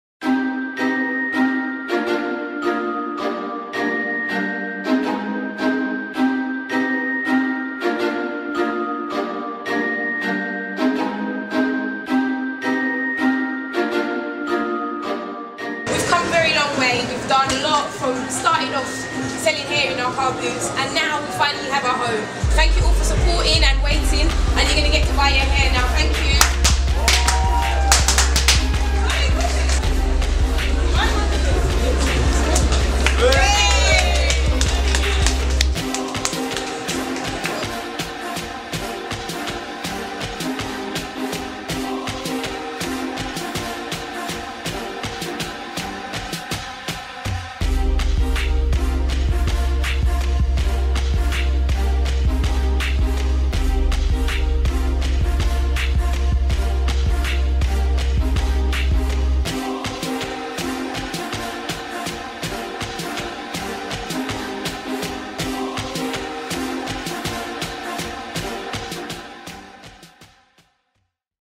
Beyond Hair Grand Opening
September 2013 saw the Grand opening of our Boutique. Take a peek of just how the day went down.